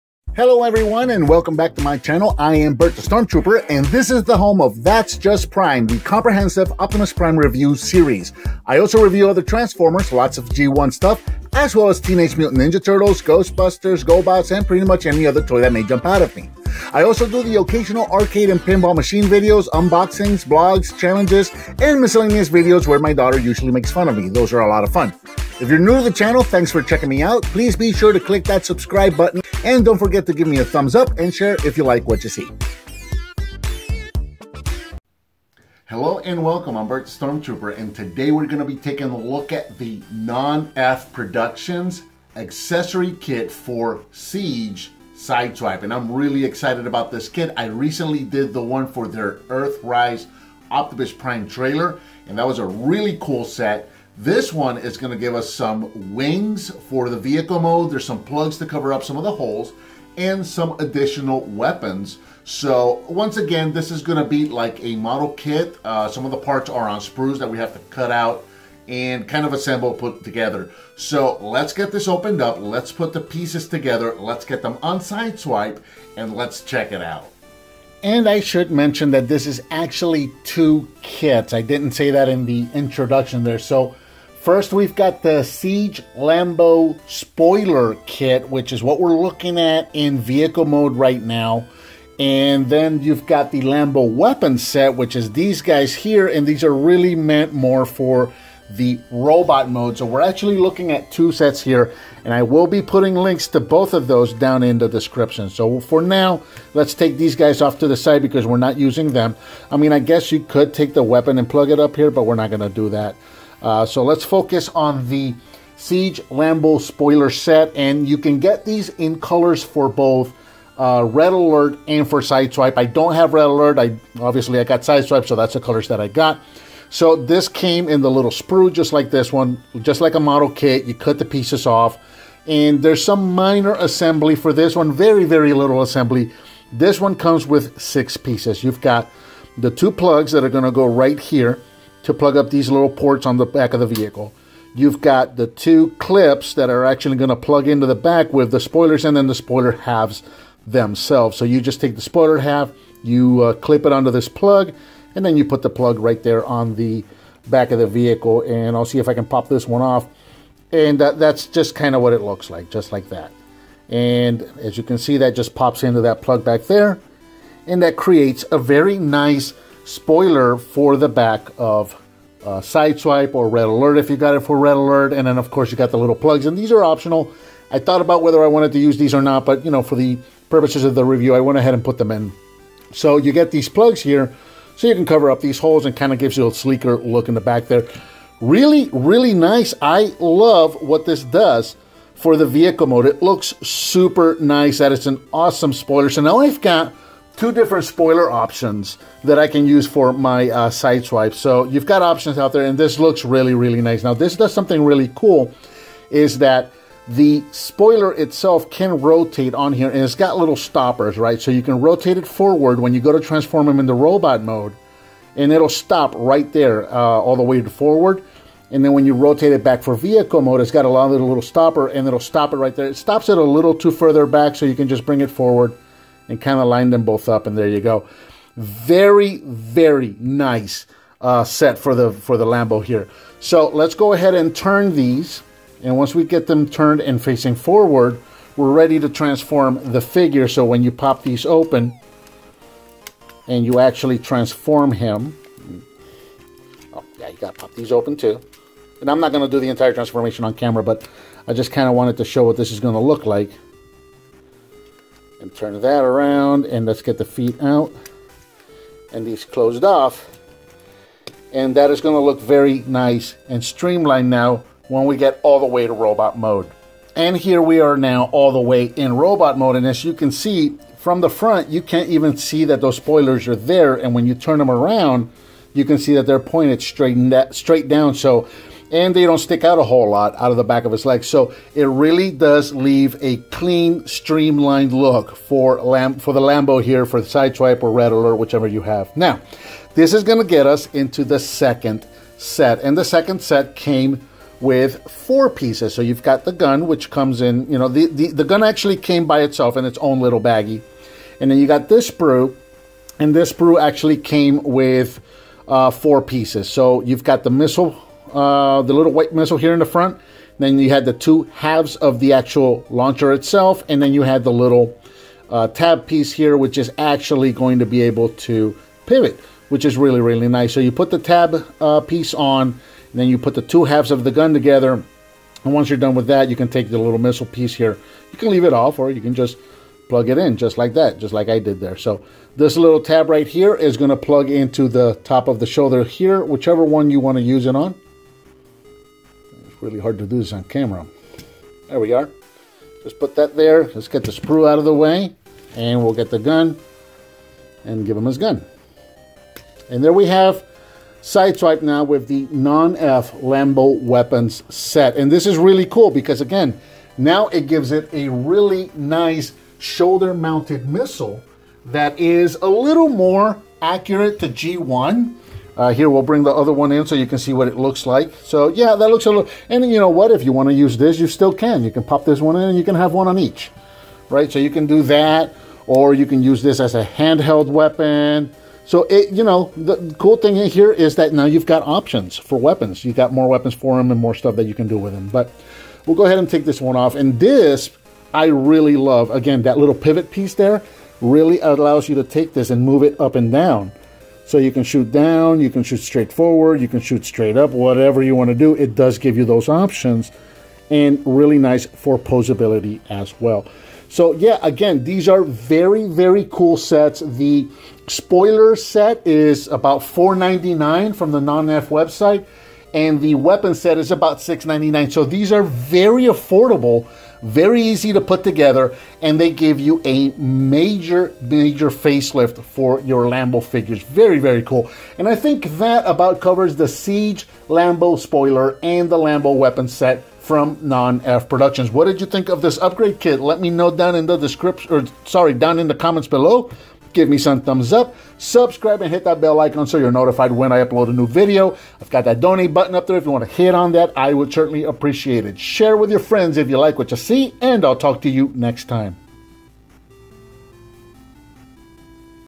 NONNEF Productions Siege LAMBO SIDESWIPE SPOILER & WEAPONS SETS! Bert the Stormtrooper Reviews!
Hello and welcome! Today we're taking a look at the NONNEF Productions Siege LAMBO (SIDESWIPE) SPOILER & WEAPONS SETS!

Get these set here:
SIEGE LAMBO SPOILER: